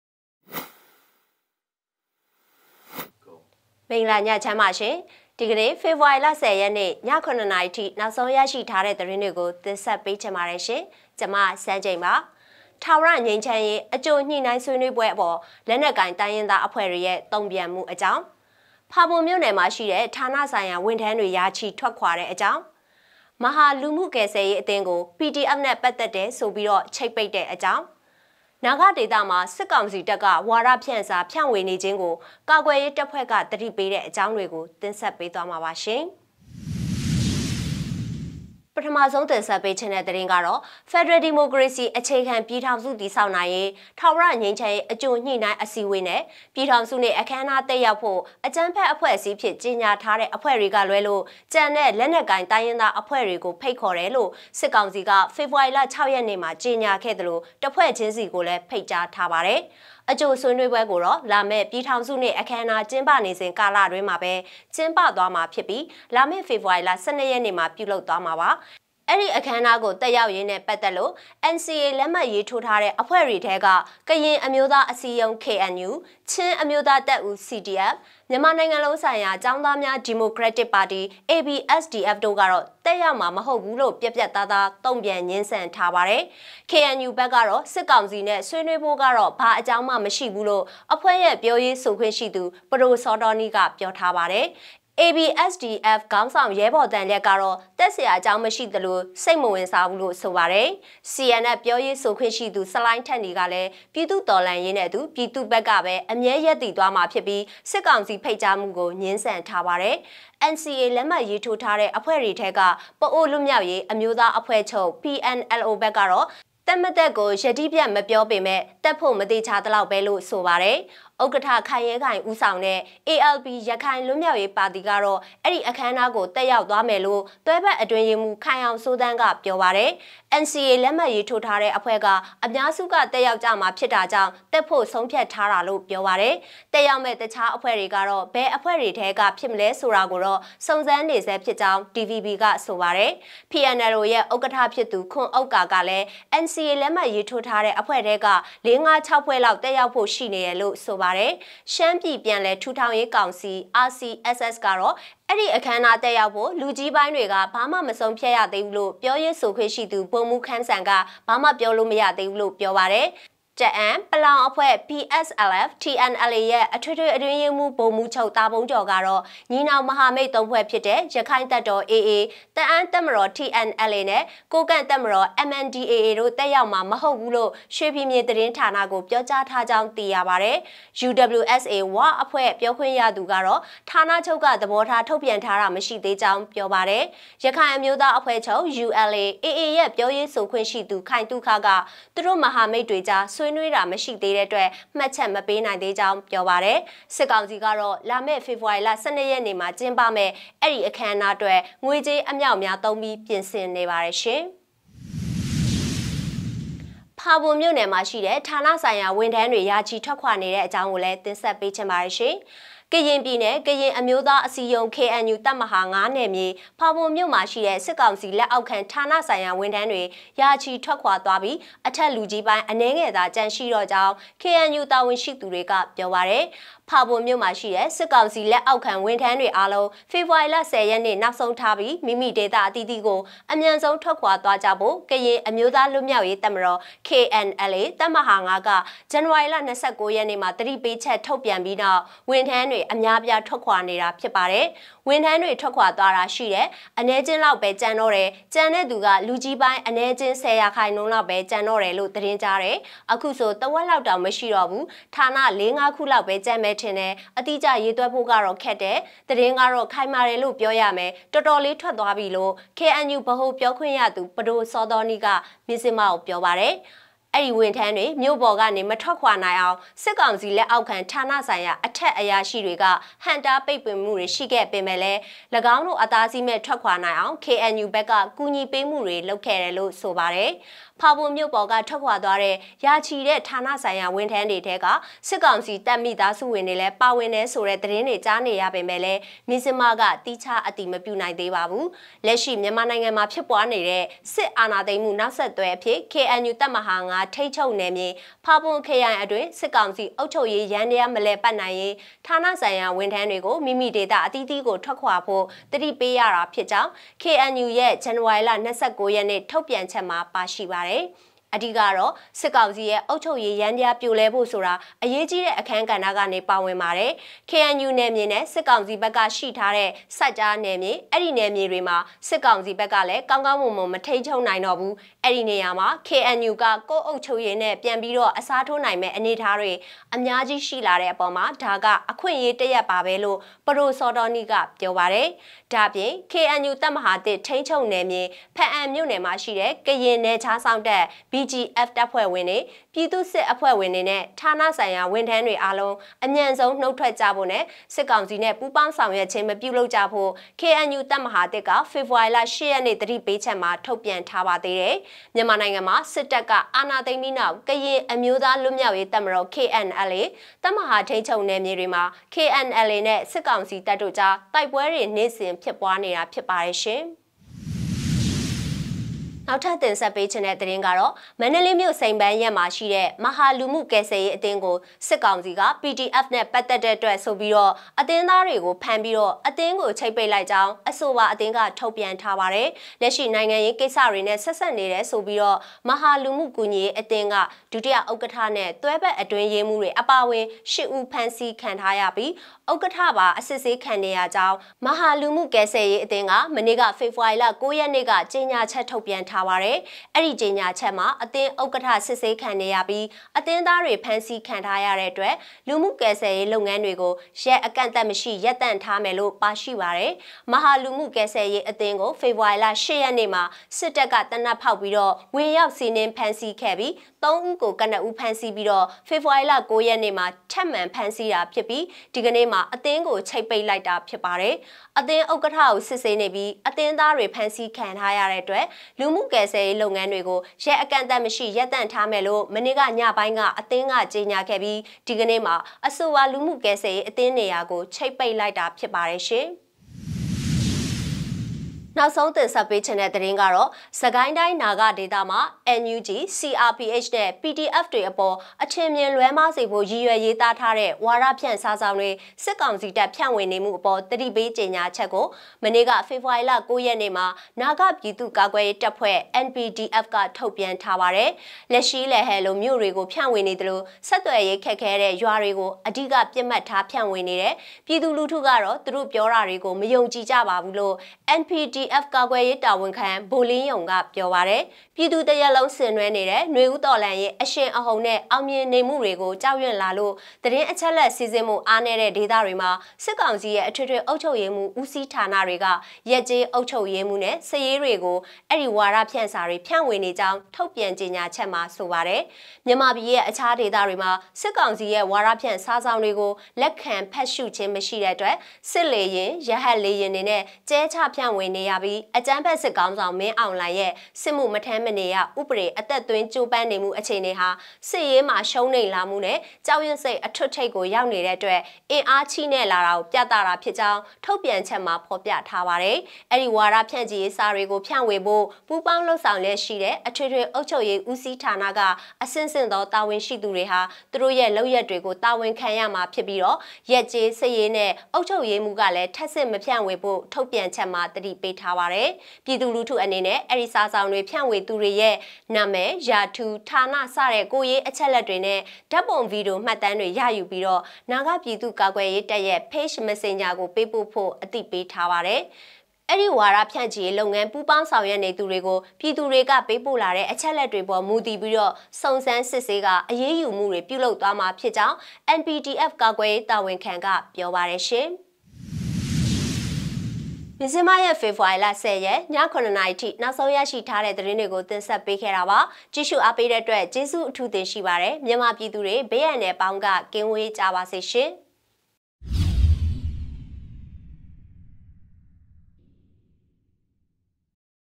ဖေဖော်ဝါရီလ ၁၀ ရက်နေ့၊ ည ၇ နာရီအထိ နောက်ဆုံးရသတင်းများ
00:00 - Intro

00:36 - ထာဝရငြိမ်းချမ်းရေး အကြိုညှိနှိုင်းဆွေးနွေးအပေါ် လက်နက်ကိုင်တိုင်းရင်းသားအဖွဲ့များ၏ တုံ့ပြန်မှု

03:25 - ဖာပွန်မြို့ရှိ  ဌာနဆိုင်ရာဝန်ထမ်းများ ရာချီထွက်ခွာ

06:07 - မဟာ လူမှုကယ်ဆယ်ရေးအသင်းကို PDFနှင့်ပတ်သက်ဟုဆိုကာ ချိတ်ပိတ်

07:20 - နာဂဒေသတွင် စစ်ကောင်စီတပ်က ဝါဒဖြန့်စာ ဖြန့်ဝေနေခြင်းကို ကာကွယ်ရေးတပ်ဖွဲ့သတိပေး

Photo - Source From Citizen Journalist
Mizzima